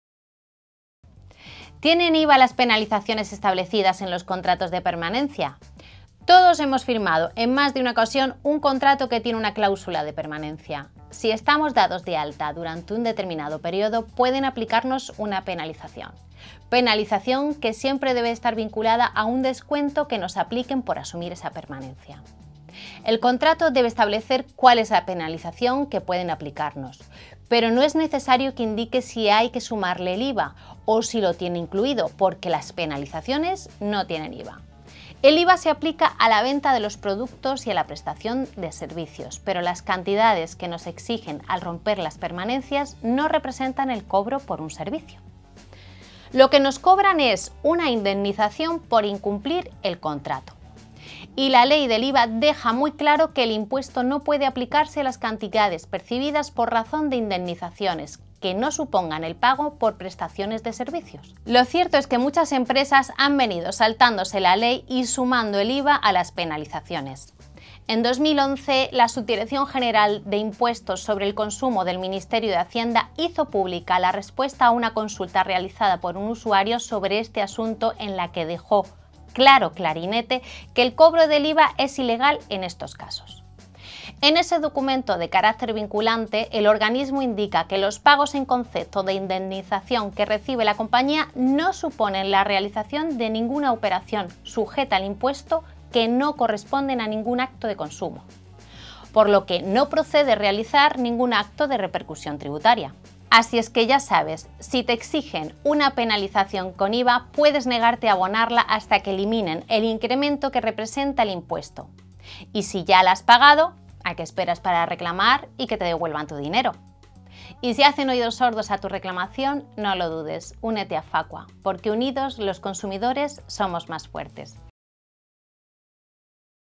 Penalizaciones abusivas al romper un compromiso de permanencia | En Ocasiones Veo Fraudes #20
Cuidado con las penalizaciones abusivas al romper un compromiso de permanencia. Y ojo, no llevan IVA.